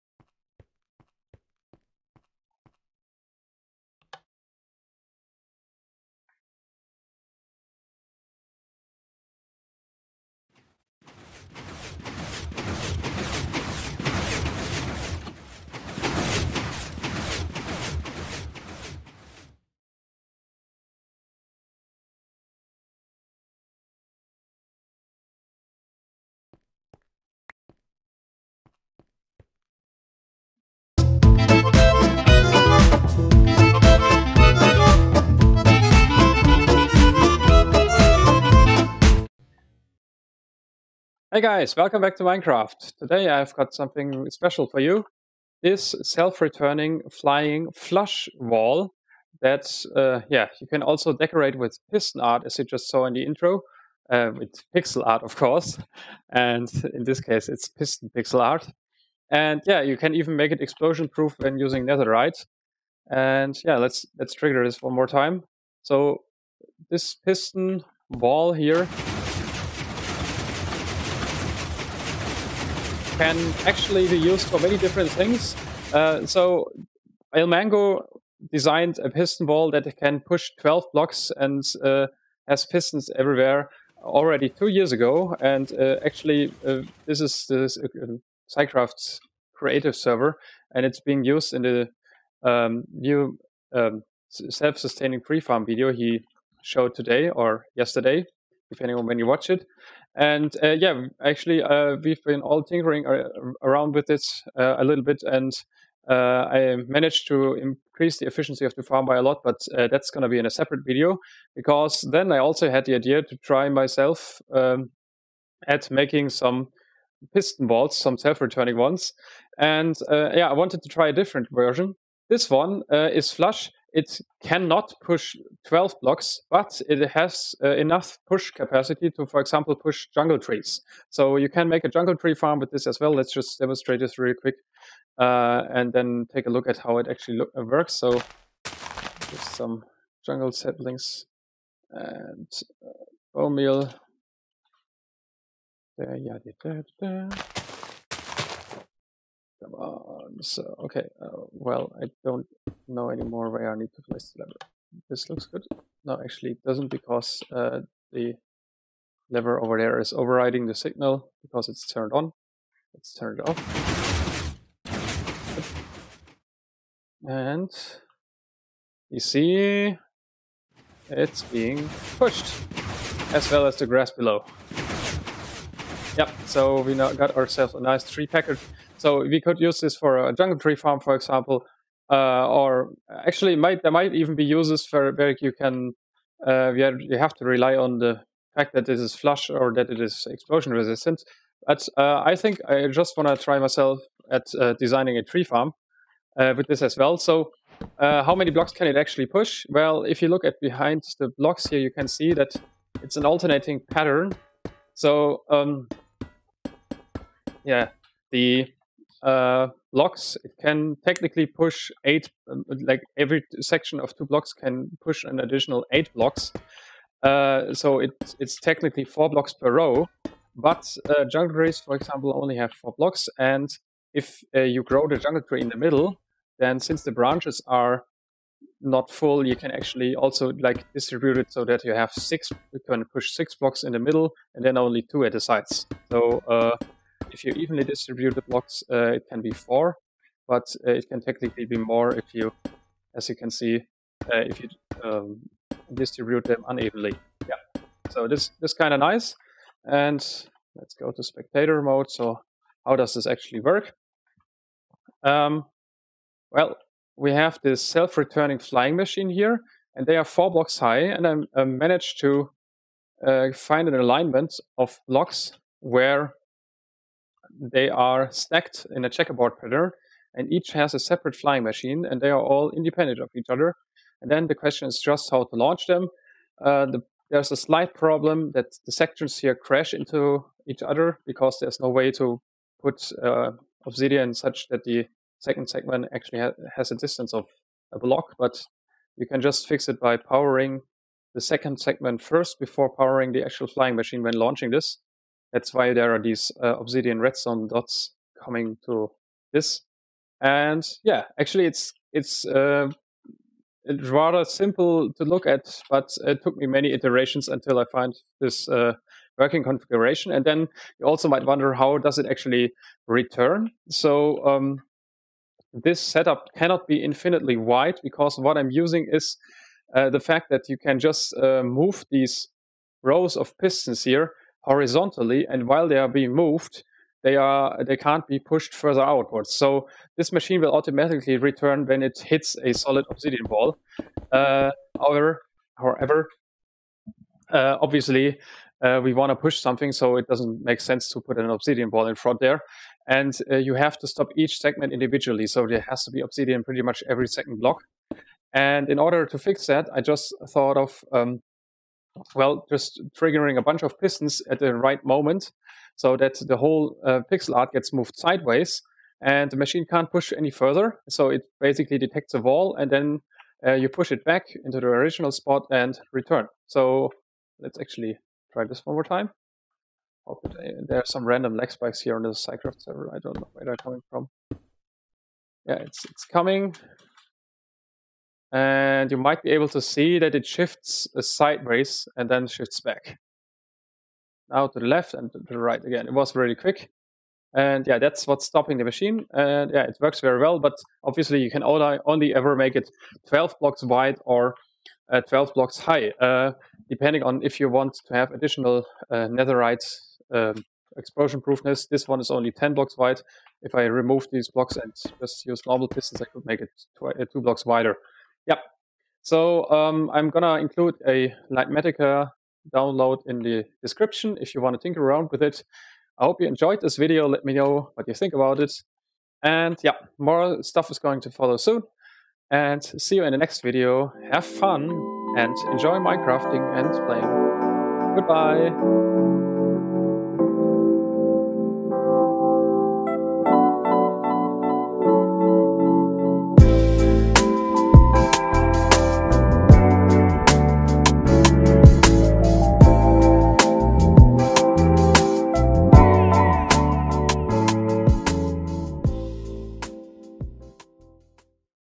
Self Returning Flush Piston Pusher
This flush wall can push up to 8 wide objects depending on their orientation!
It can also be made explosion proof using netherite. There are many possible applications for this design, i.e. cool tree farms.